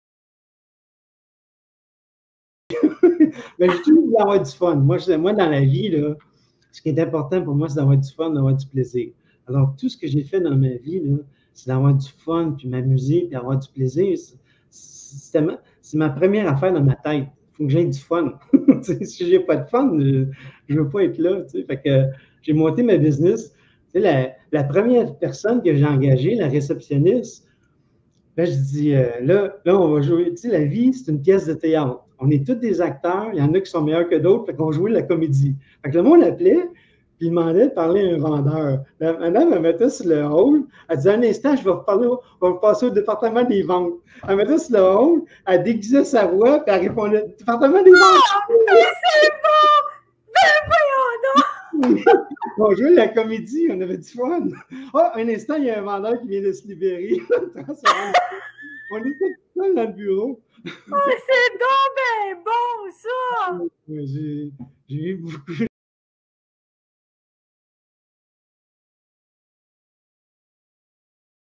Meilleur moment comique de mon entrevue avec Sophie. Marcel Richard Junior. 07/2024
Meilleur moment comique de mon entrevue avec Sophie. Pour moi la vie c’est une pièce de théâtre, nous sommes tous des acteurs, il y en a qui sont meilleurs que d’autres. Lollll La version intégral de l’entrevue est disponible.